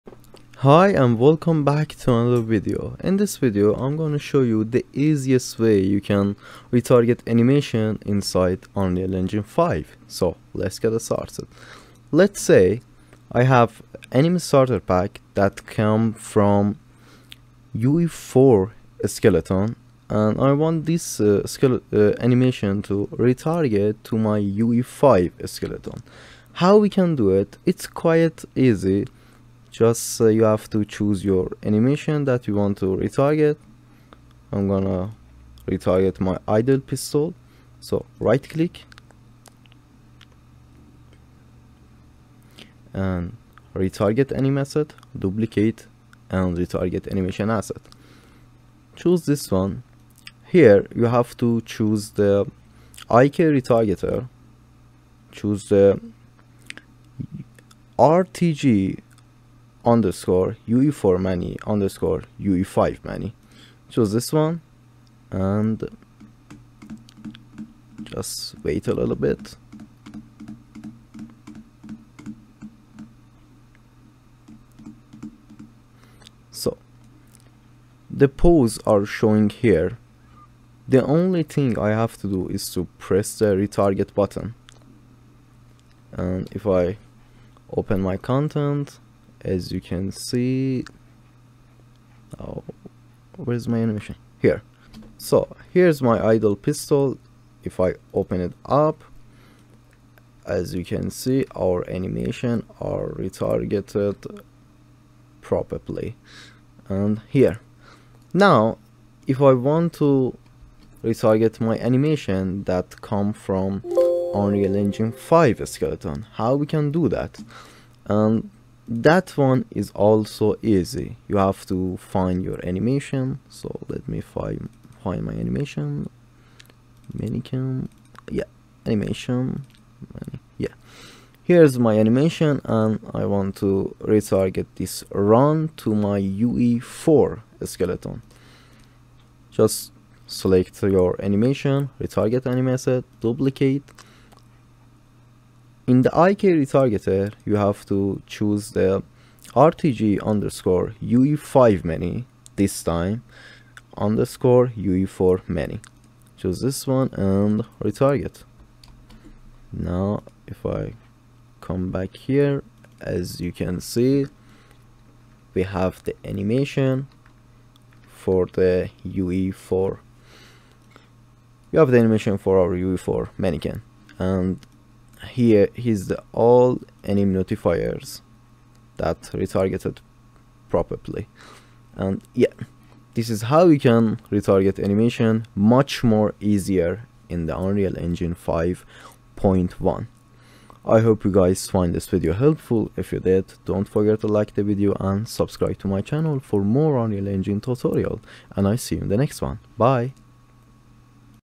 How To Retarget Animations In Unreal Engine 5.1
Hi and welcome to another video did you retargeting animations in unreal engine 5.1 is so much more easy ? with this mothed you can retarget animations from ue4 skeleton to ue5 skeleton 😇
Chapters
00:00 - intro
00:16 - Retargeting animations tutorial
03:04 -  Outro